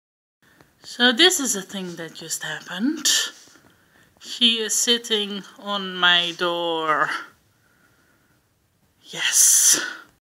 Cat on door
My cat BellaDonna decided that she needed to sit on top of my door.